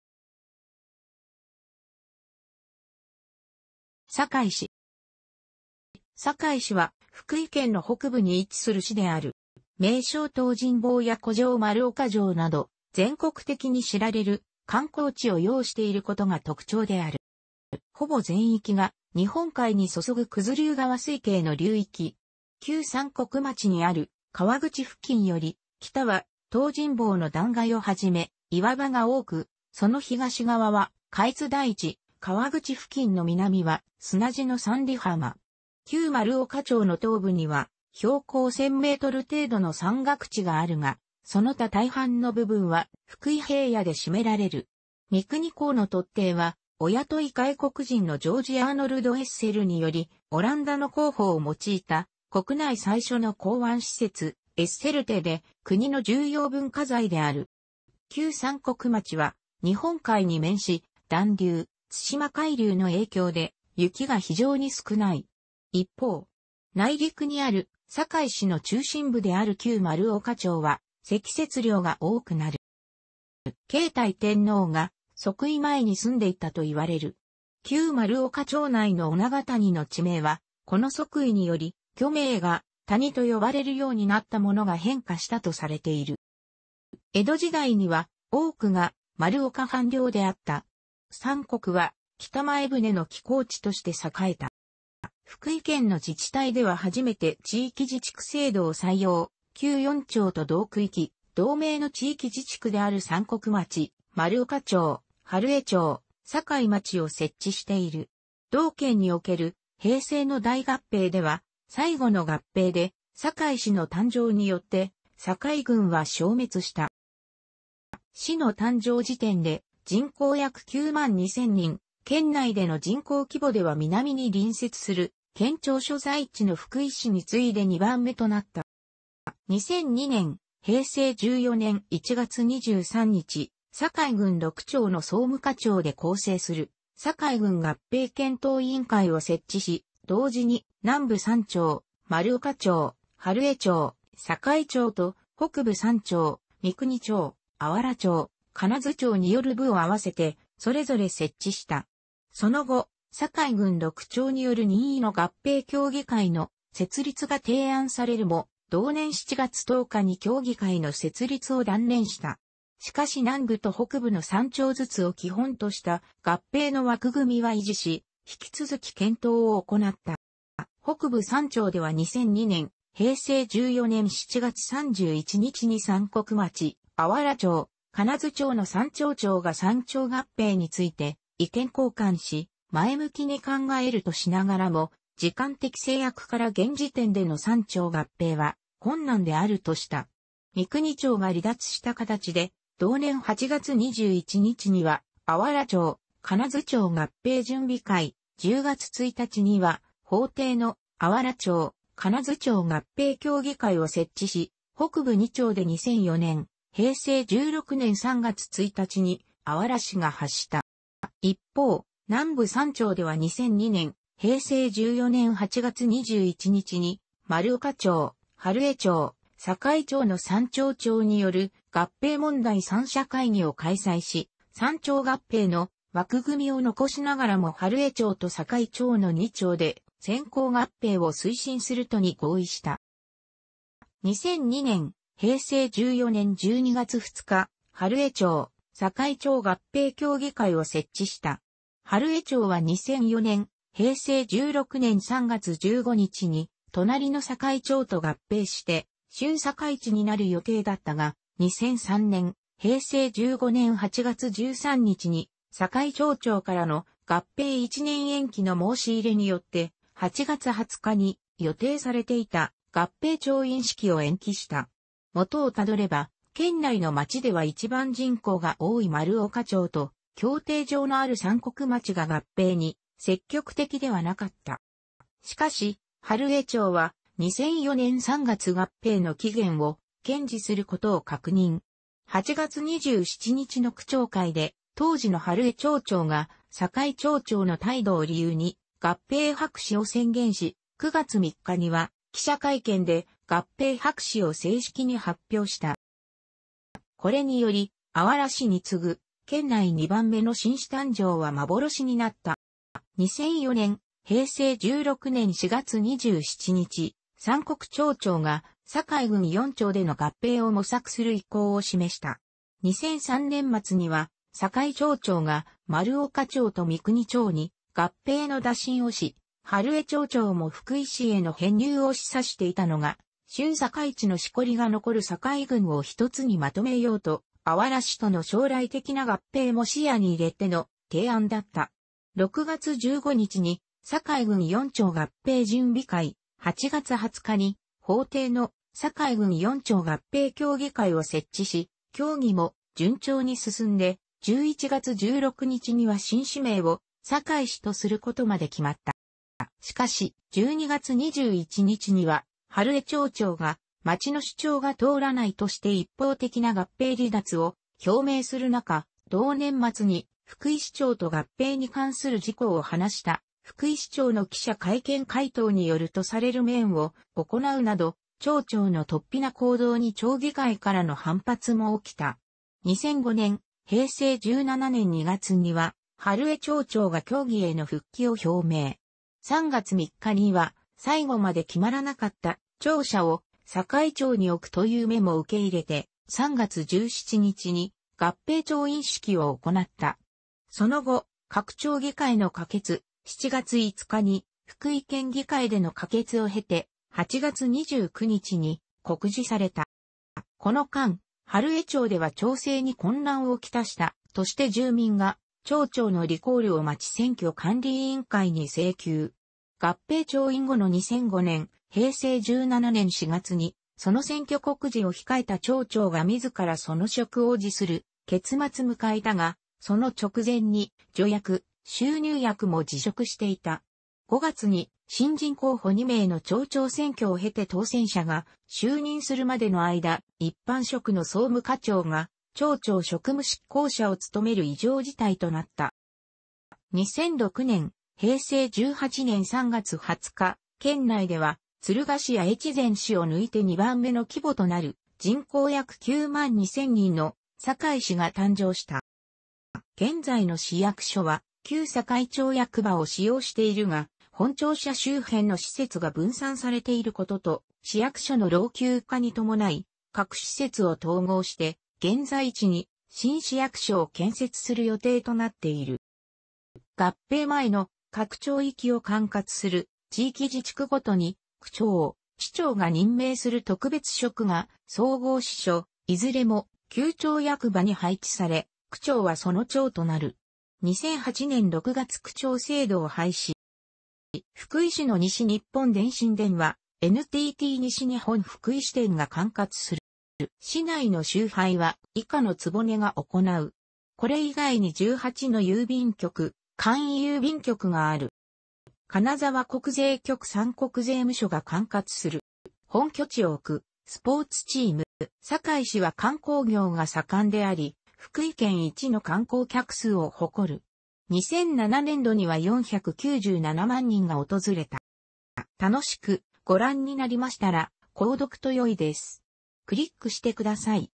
坂井市
坂井市（さかいし）は、福井県の北部に位置する市である。

名勝東尋坊や古城丸岡城など、全国的に知られる観光地を擁していることが特徴である。

ほぼ全域が日本海に注ぐ九頭竜川水系の流域。旧三国町にある河口付近より北は東尋坊の断崖をはじめ岩場が多く、その東側は加越台地、河口付近の南は砂地の三里浜。旧丸岡町の東部には標高1000m程度の山岳地があるが、その他大半の部分は福井平野で占められる。
三国港の突堤は、お雇い外国人のジョージ・アーノルド・エッセルにより、オランダの工法を用いた国内最初の港湾施設（エッセル堤）で、国の重要文化財である。

旧三国町は日本海に面し、暖流（対馬海流）の影響で雪が非常に少ない。一方。内陸にある坂井市の中心部である旧丸岡町は積雪量が多くなる。

継体天皇が即位前に住んでいたといわれる。旧丸岡町内の女形谷（おながたに）の地名はこの即位により「御名が谷」と呼ばれるようになったものが変化したとされている。

江戸時代には多くが丸岡藩領であった。三国は北前船の寄港地として栄えた。

福井県の自治体では初めて地域自治区制度を採用、旧4町と同区域・同名の地域自治区である三国町、丸岡町、春江町、坂井町を設置している。同県における「平成の大合併」では最後の合併で、坂井市の誕生によって坂井郡は消滅した。

市の誕生時点で人口約9万2000人、県内での人口規模では南に隣接する県庁所在地の福井市に次いで2番目となった。

2002年（平成14年）1月23日、坂井郡6町の総務課長で構成する「坂井郡合併検討委員会」を設置し、同時に南部3町（丸岡町・春江町・坂井町）と北部3町（三国町・芦原町・金津町）による部会を併せて、それぞれ設置した。その後、坂井郡6町による任意の合併協議会の設立が提案されるも、同年7月10日に協議会の設立を断念した。しかし南部と北部の3町ずつを基本とした合併の枠組みは維持し、引き続き検討を行った。

北部3町では2002年（平成14年）7月31日に三国町、芦原町、金津町の3町長が3町合併について意見交換し、前向きに考えるとしながらも時間的制約から現時点での3町合併は困難であるとした。三国町が離脱した形で同年8月21日には「芦原町・金津町合併準備会」、10月1日には法定の「芦原町・金津町合併協議会」を設置し、北部2町で2004年（平成16年）3月1日に「あわら市」が発足した。

一方、南部3町では2002年（平成14年）8月21日に丸岡町、春江町、坂井町の3町長による合併問題三者会議を開催し、3町合併の枠組みを残しながらも春江町と坂井町の2町で先行合併を推進することに合意した。

2002年（平成14年）12月2日、「春江町・坂井町合併協議会」を設置した。春江町は2004年（平成16年）3月15日に、隣の坂井町と合併して「春坂市」になる予定だったが、2003年（平成15年）8月13日に坂井町長からの「合併一年延期」の申し入れによって、8月20日に予定されていた合併調印式を延期した。（元をたどれば、県内の町では一番人口が多い丸岡町と、競艇場のある三国町が合併に積極的ではなかった）

しかし、春江町は「2004年3月合併」の期限を堅持することを確認。8月27日の区長会で、当時の春江町長が坂井町長の態度を理由に「合併白紙」を宣言し、9月3日には記者会見で合併白紙を正式に発表した。

これにより、あわら市に次ぐ県内二番目の新市誕生は幻になった。

2004年（平成16年）4月27日、三国町長が坂井郡4町での合併を模索する意向を示した。2003年末には坂井町長が丸岡町と三国町に合併の打診をし、春江町長も福井市への編入を示唆していたのだが、春坂市のしこりが残る坂井郡を一つにまとめようと、あわら市との将来的な合併も視野に入れての提案だった。6月15日に「坂井郡四町合併準備会」、8月20日に法定の「坂井郡四町合併協議会」を設置し、協議も順調に進んで、11月16日には新市名を「坂井市」とすることまで決まった。

しかし、12月21日には春江町長が「町の主張が通らない」として一方的な合併離脱を表明するなか、同年末に福井市長と合併に関する事項を話した（福井市長の記者会見回答による）とされる面談を行うなど、町長の突飛な行動に町議会からの反発も起きた。2005年（平成17年）2月には春江町長が協議への復帰を表明。3月3日には、最後まで決まらなかった「庁舎を坂井町に置く」という項目も受け入れて、3月17日に合併調印式を行った。その後、...